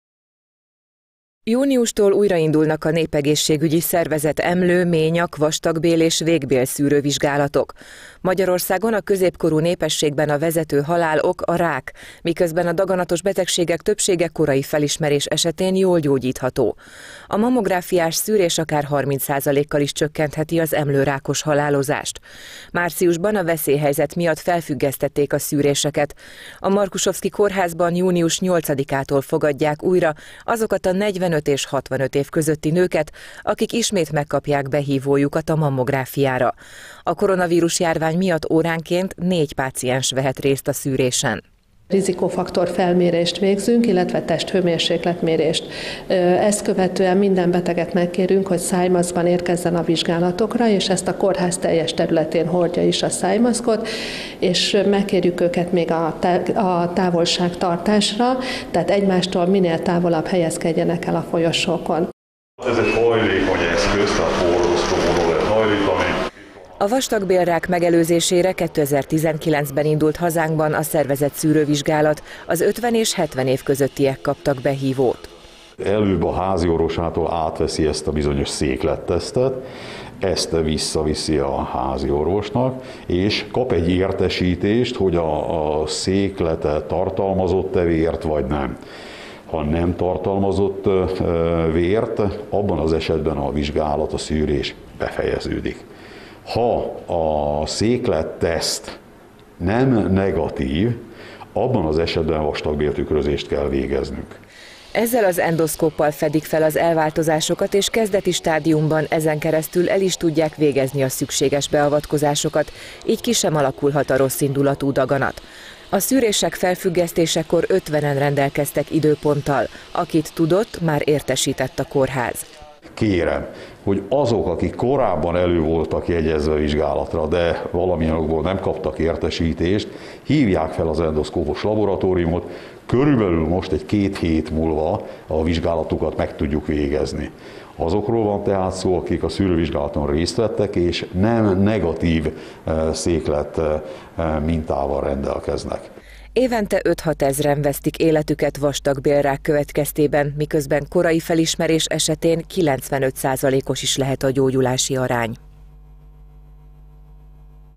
Indulnak a szűrővizsgálatok
Márciusban a veszélyhelyzet miatt felfüggesztették a szűréseket. A Markusovszky Kórházban június 8-tól fogadják újra azokat a 45-65 év közötti nőket, akik ismét megkapják behívójukat a mammográfiára. A koronavírus-járvány miatt óránként 4 páciens vehet részt a szűrésen.